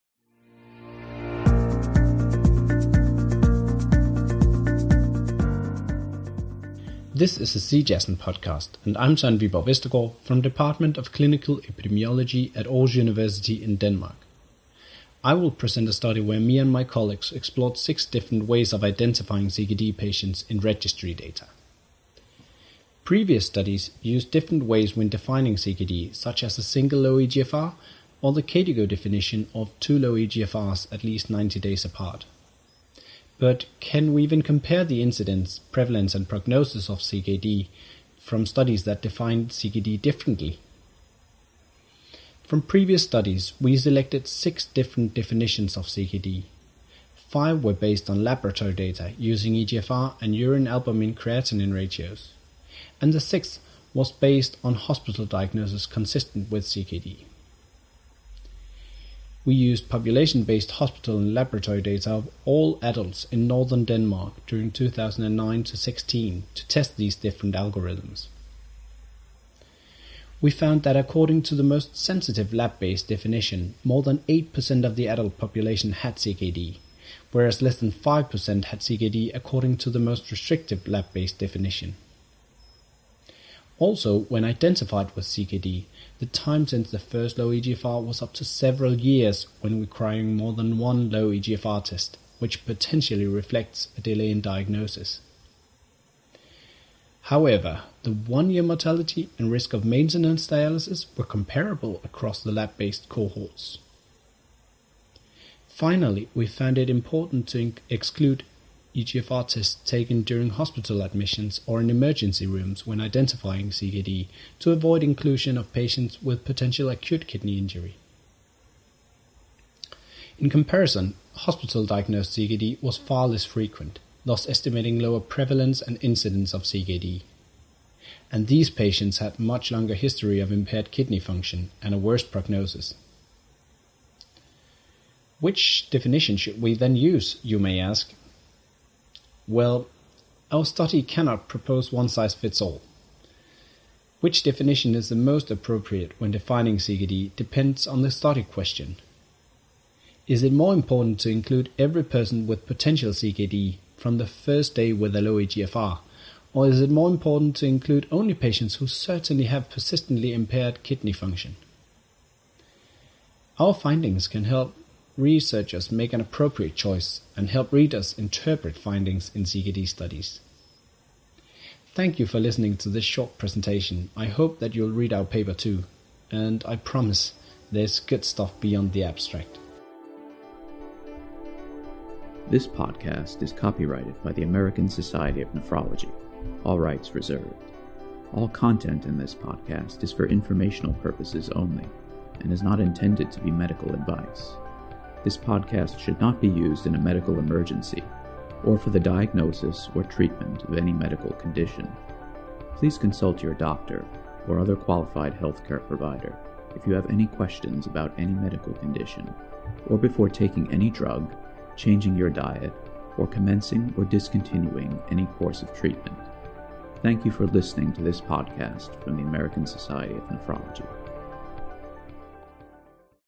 Algorithms to Identify Chronic Kidney Disease
Dr. Søren Viborg Vestergaard provides a summary of his study "Identification of Patients with CKD in Medical Databases," on behalf of his colleagues.